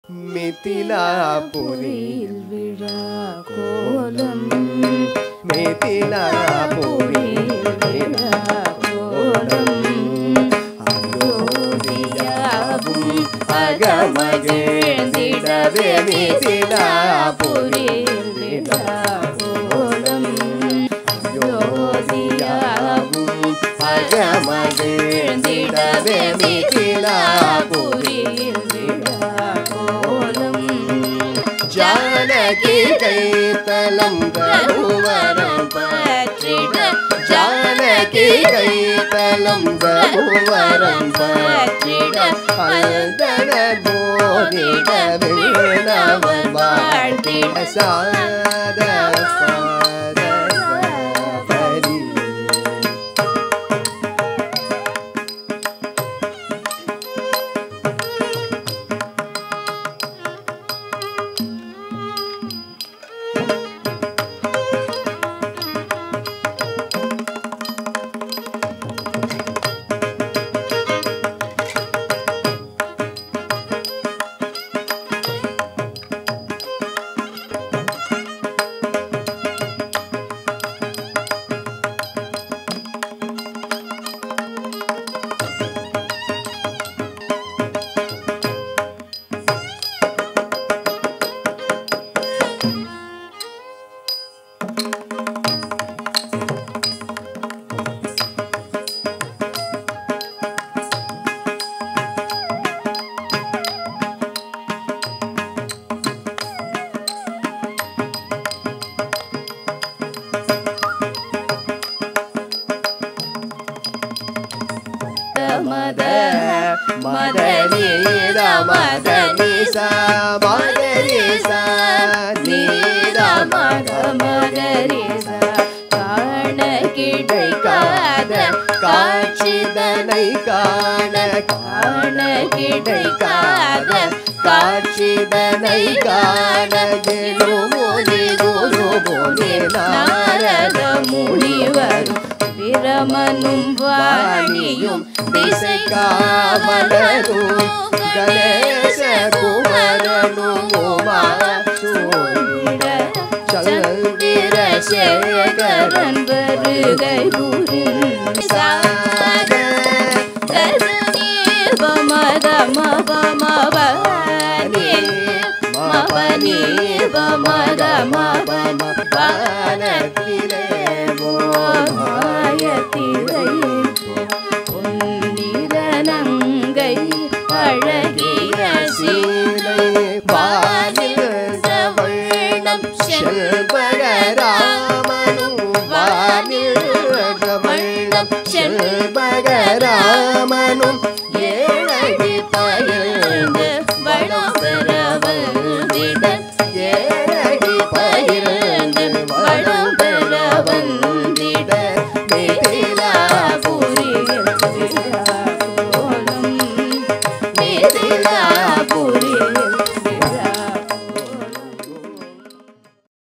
UPAVARNANA
It's a innovative idea by TAPAS Academy to incorporate UPAVARANA in usual margam.
This particular UPAVARNANA describes the arrangements, decorations carried out preceding to Rama-Sita Kalyanam.
This piece is set to Raagam- Raagamaalika (Vasantha-mohanam-amruthavarshini) in Thalam - aadhi.
Choerographed by Smt. Mahalakshmi Ashwin.
Lyrics by Ashwin Kumar Iyer.
Set to tune by Shri. P. B. Sri Rangachaari.